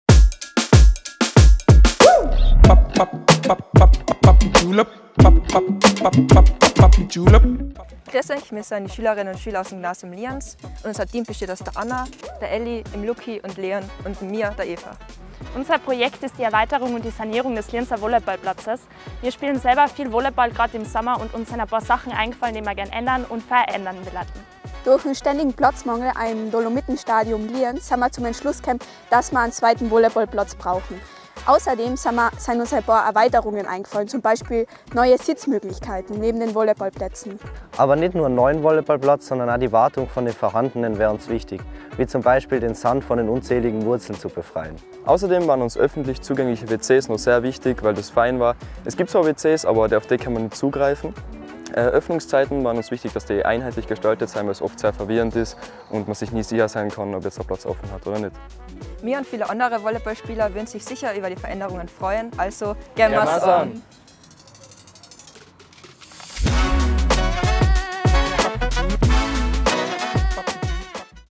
gemmason -Challenge: Einreichung 6 von 24 - ENTWICKLUNGSRAUM DER TALENTE
„Volleyballplatz“
von Lukas K., Peter S., Leon S., Eva B., Anna O., Sophie B., Elena U., David S., Ruben S., Ben T.

Lange Schlangen beim einzigen Volleyballplatz in Lienz, zu wenig Sitzmöglichkeiten im Schatten und zugesperrt wird auch zu früh - diesem Problem wollen die Schüler:innen des Gymnasiums Lienz Abhilfe schaffen. Wie sie das machen wollen und was sie sonst noch für den Lienzer Volleyballplatz geplant haben, erfährst du in diesem Video! 🏐

© ARGE Vordenken für Osttirol/Bachmann E.